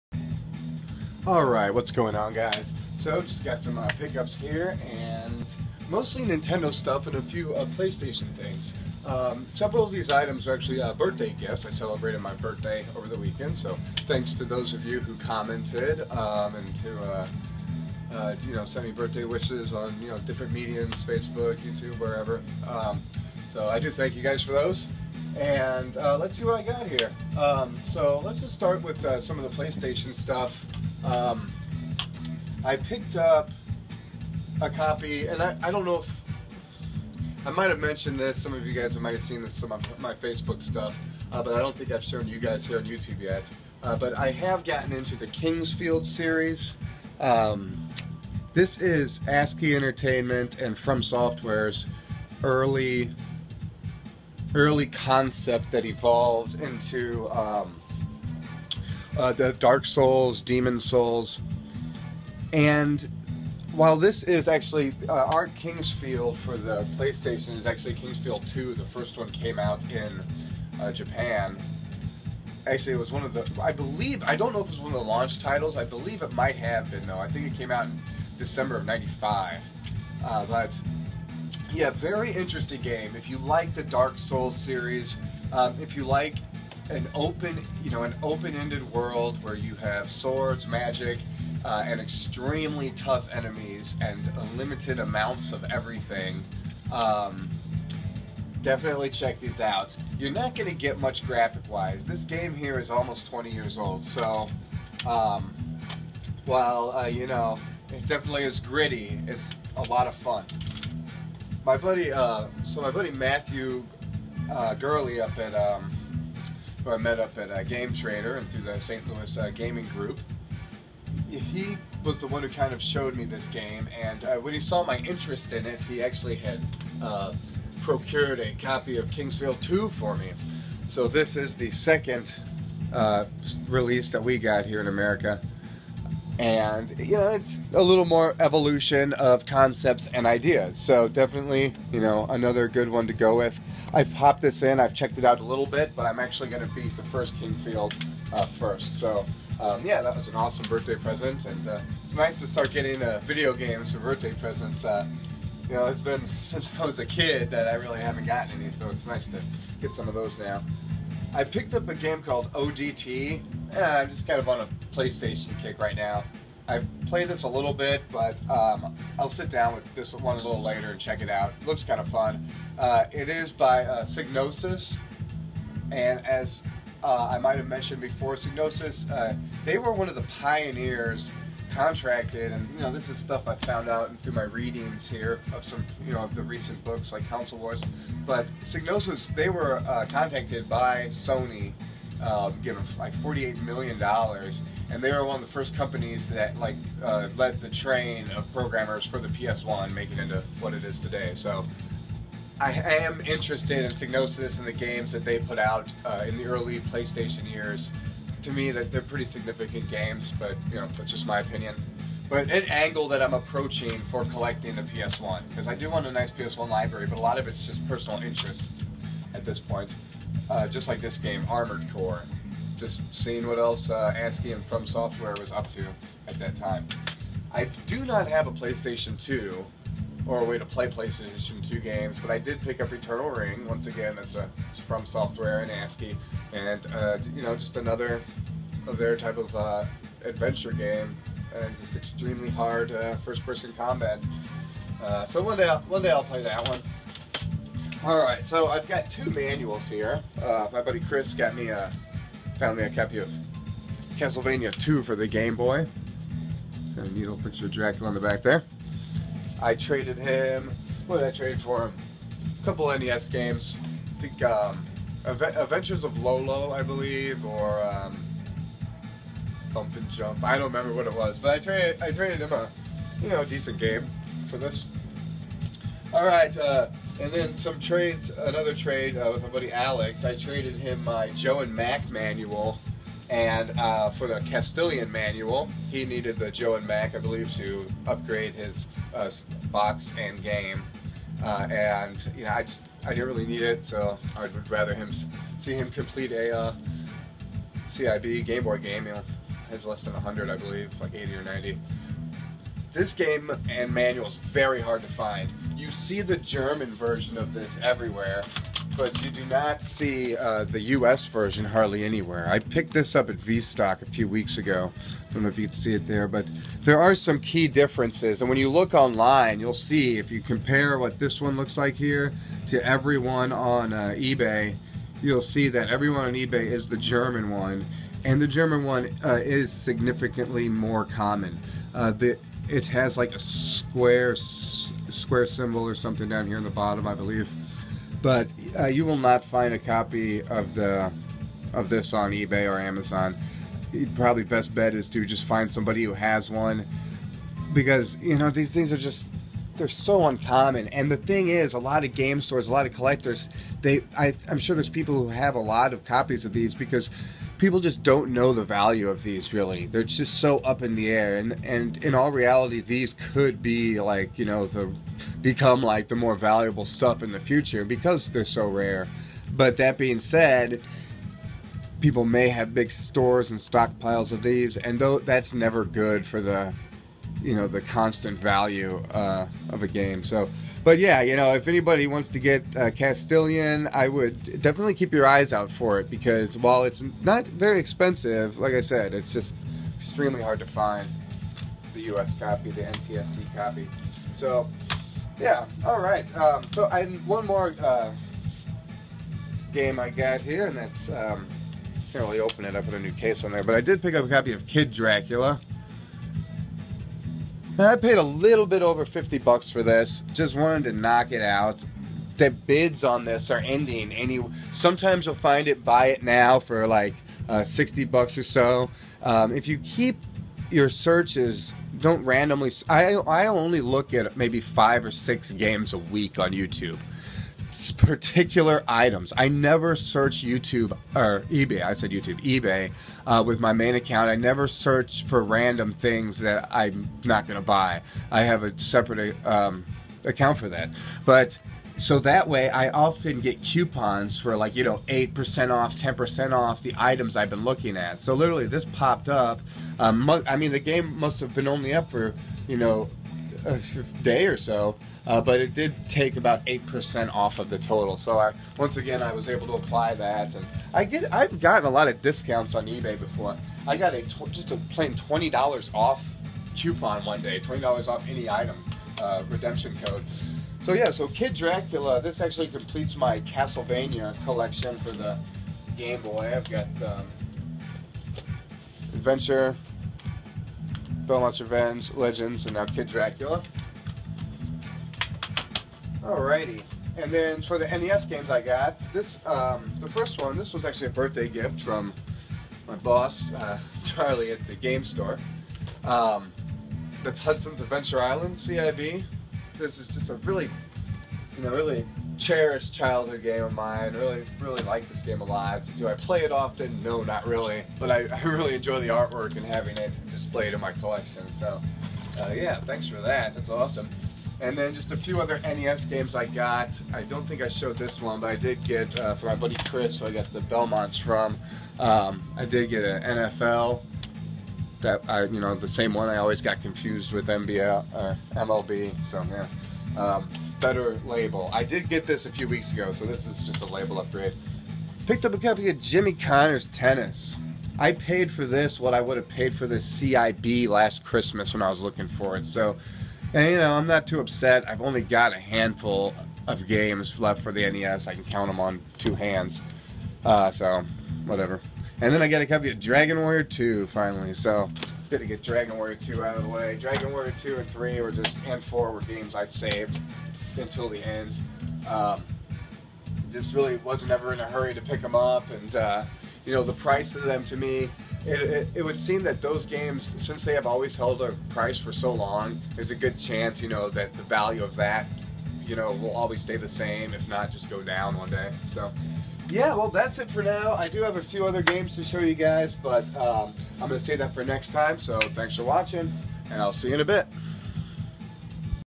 Pickups Gifts and Trades !!
Bday gifts some trades ,ps1 and nintendo pickups!